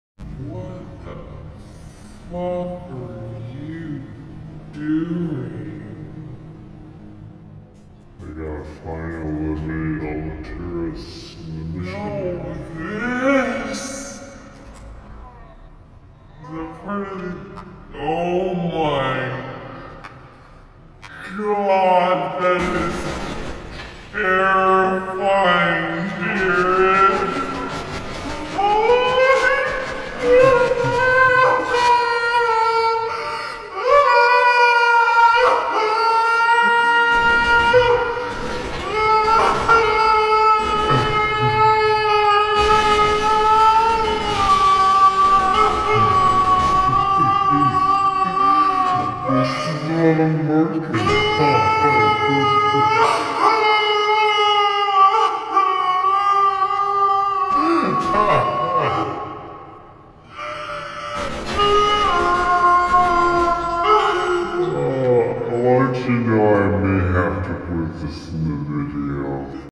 Jareed Cheet
What happens when I warp a ProJared video.